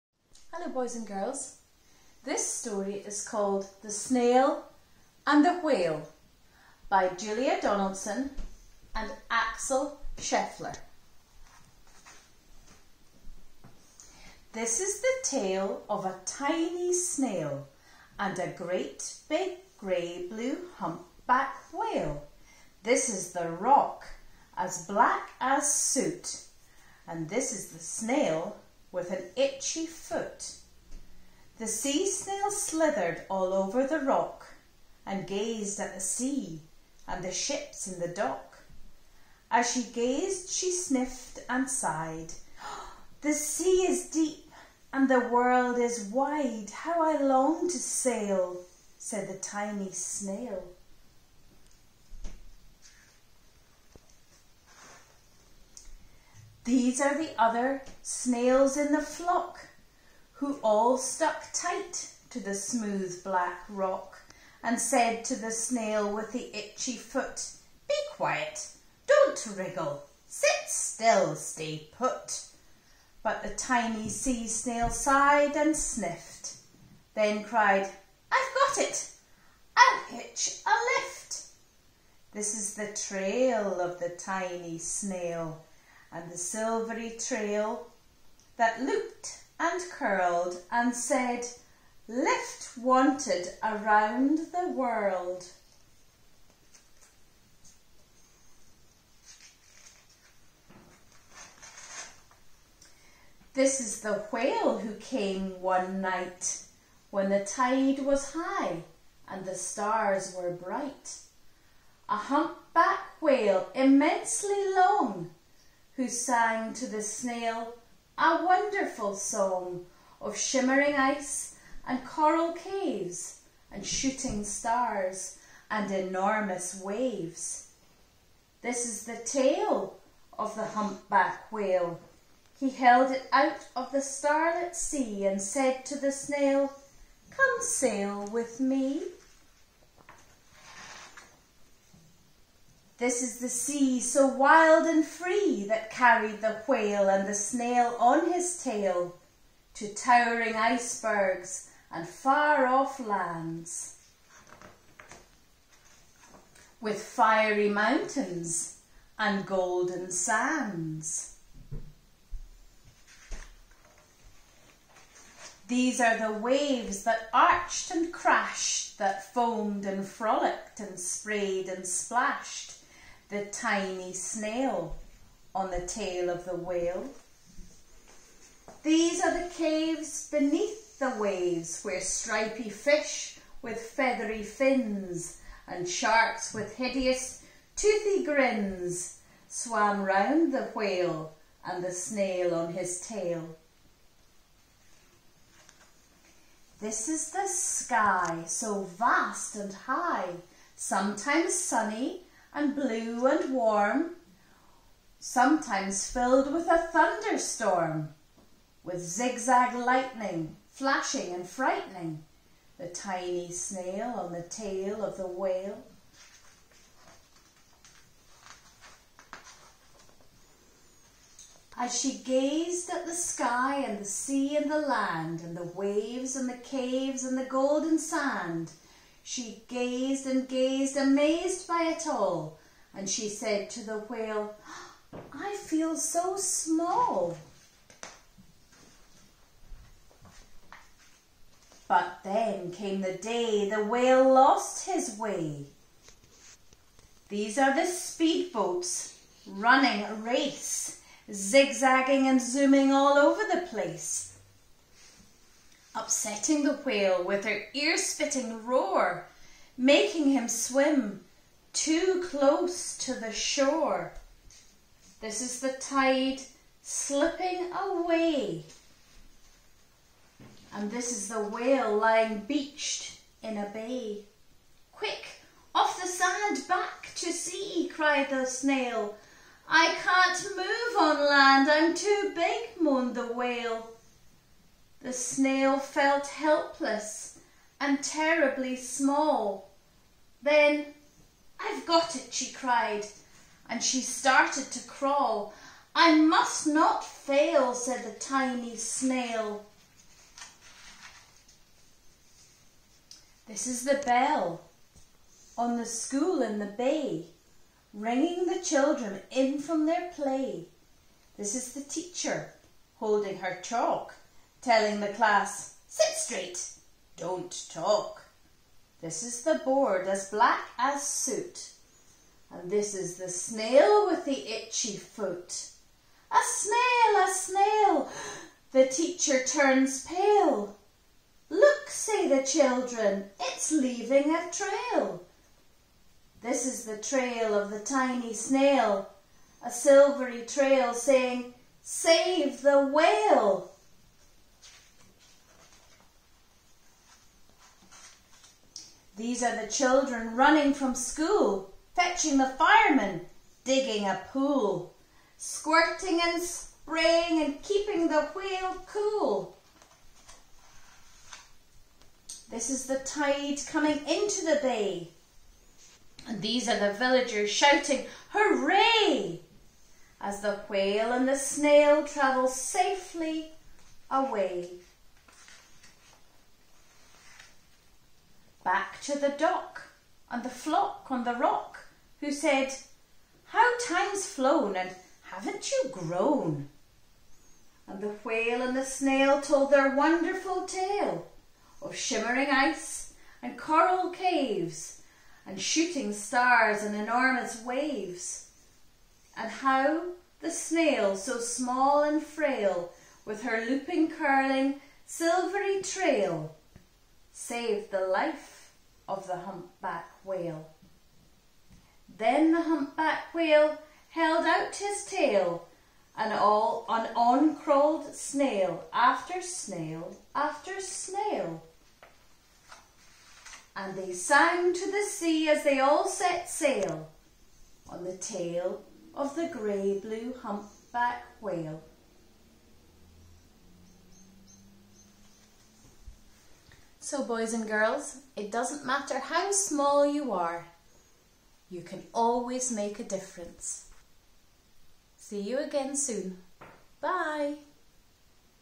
The Snail and the Whale
Listen along to Mrs. Fitzgerald reading the book 'The Snail and the Whale'.